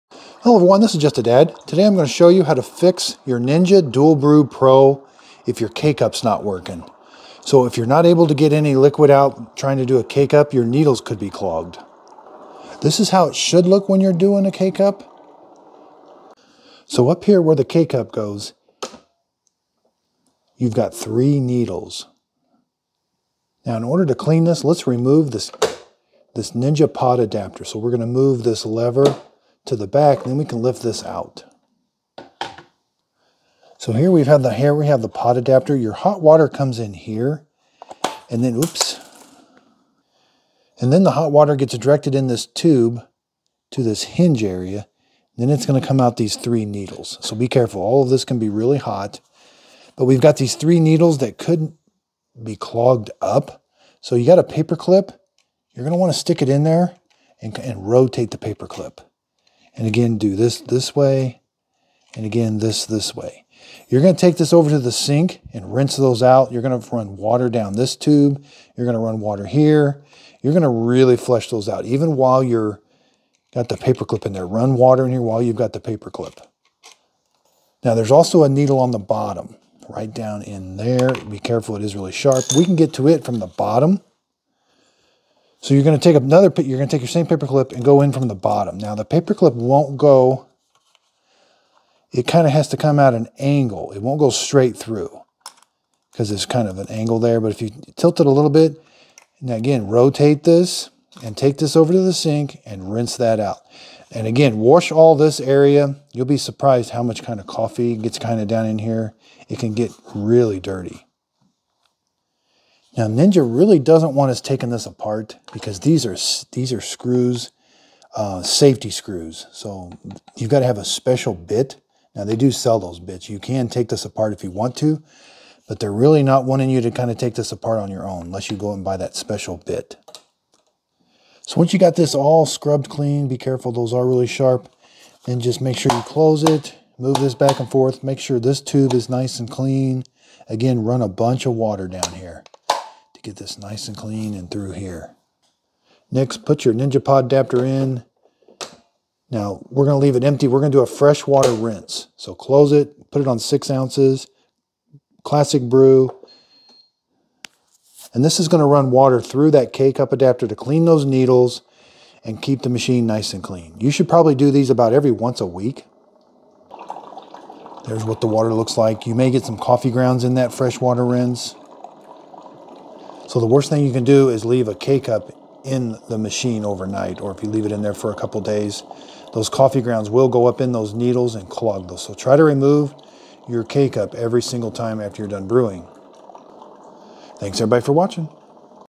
QUICK FIX Ninja DualBrew Pro CFP301 Coffee Maker NOT WORKING? CLEAN NEEDLES
How to Clean Needles and fix your Ninja CFP301 & CFP300 DualBrew Pro Specialty 12-Cup Drip Maker with Glass Carafe, Single-Serve for Coffee Pods or Grounds, with 4 Brew Styles, Frother & Separate Hot Water System.  How to remove the upper and lower needle and clean with a paper clip.  Nothing comes out when you brew a k cup? clean needles.